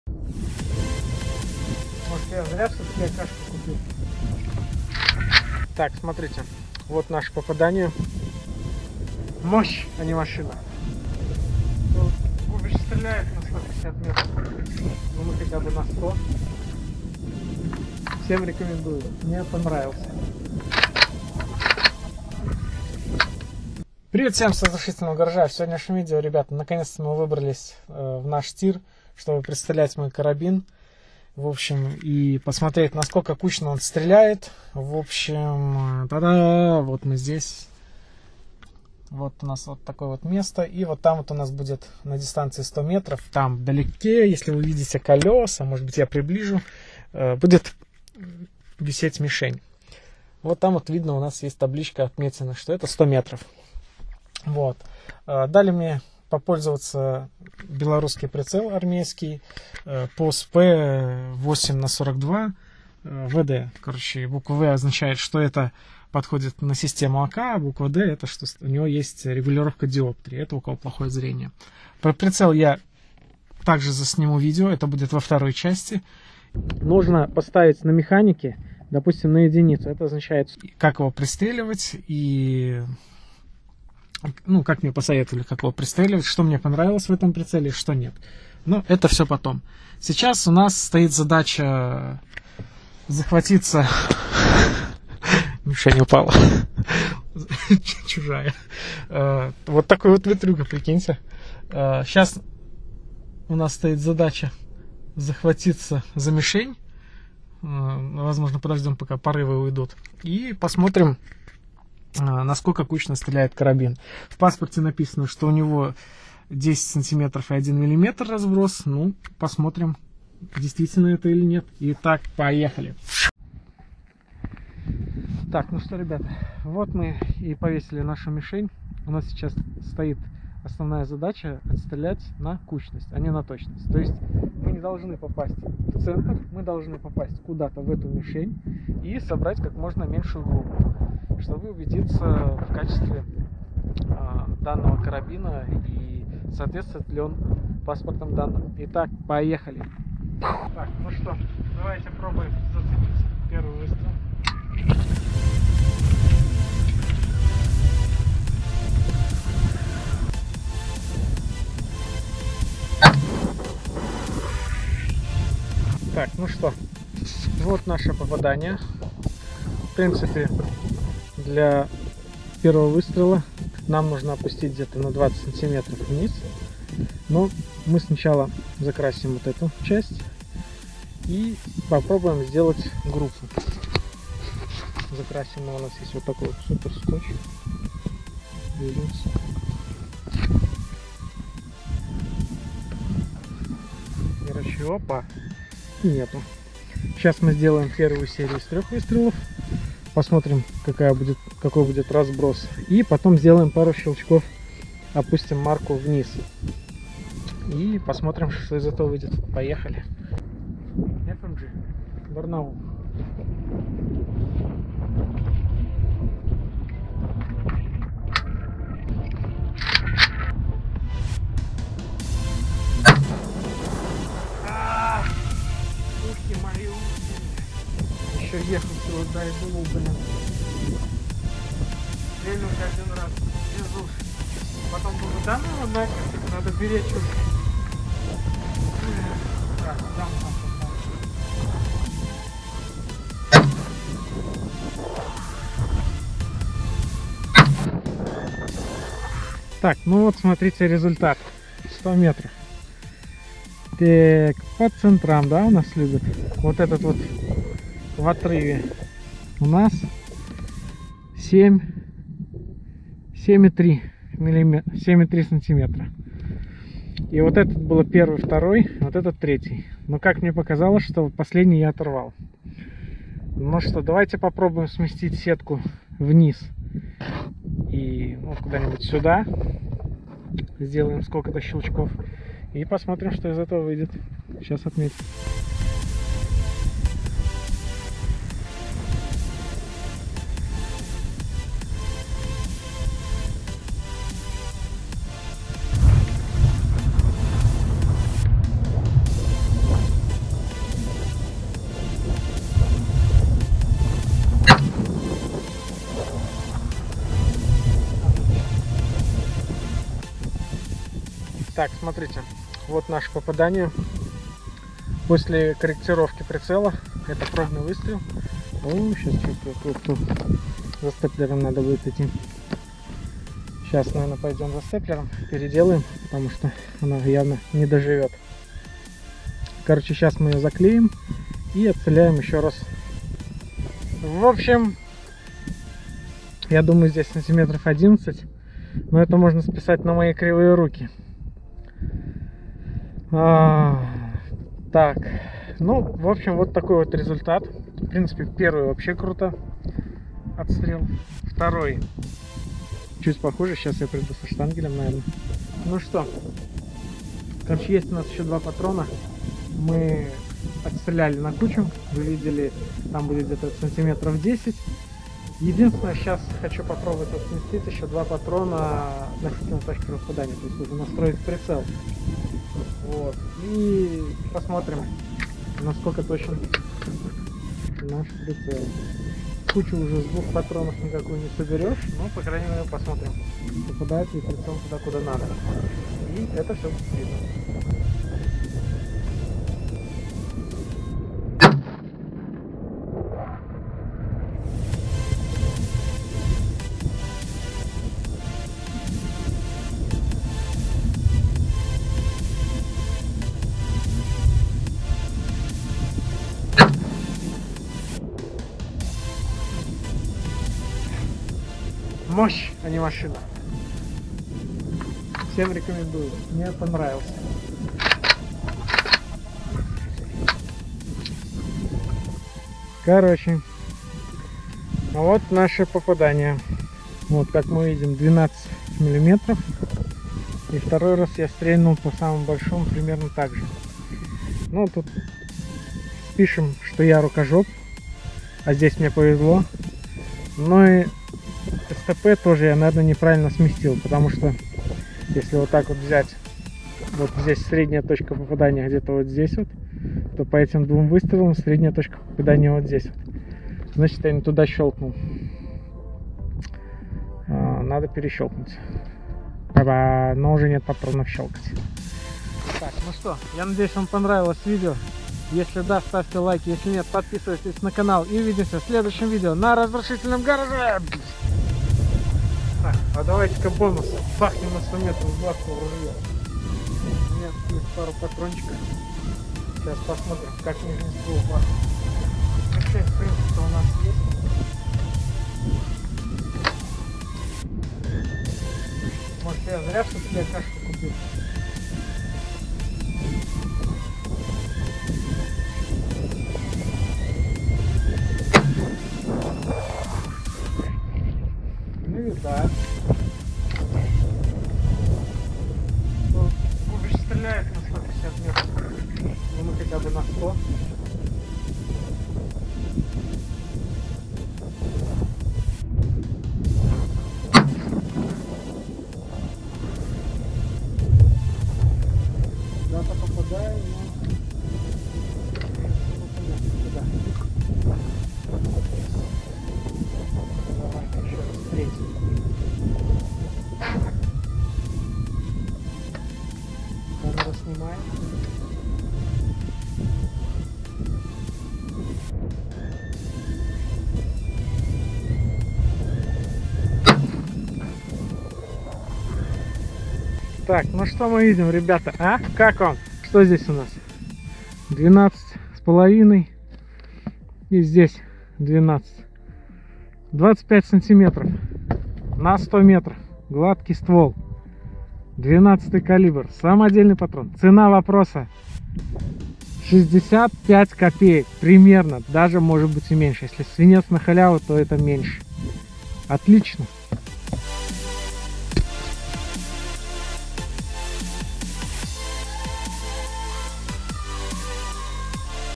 Сайга 030 5.45х39 | Отстрел на кучность на 100м | 1 часть.
В данном видео вы увидите отстрел карабина Сайга 030 в калибре 5.45х39 с оптическим прицелом ПОСП 8х42ВД армейский вариант или под армейский))) Погода немного не задалась, но как мы все знаем на охоте мы не выбираем погоду))) поэтому как я считаю размер групп  немного  меня разочаровал в начале, но потом я понял что в этих условиях возможно это даже совсем не плохой результат))) в конце будет небольшой бонус он меня вообще порадовал)))